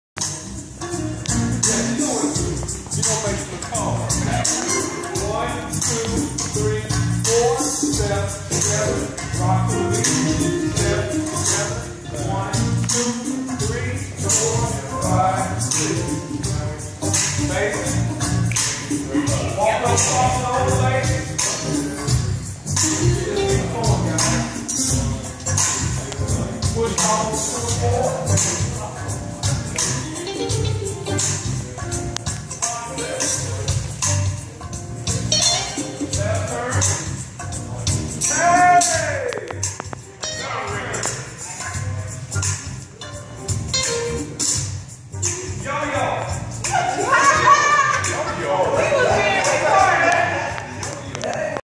Students Chicago Style Steppin Couples Basics 2
Clips From Wednesday Class At Southtown Pavilion.Students practicing together to adjust to different types of leads and varying response times from the ladies. To learn more about local classes or online lesson visit our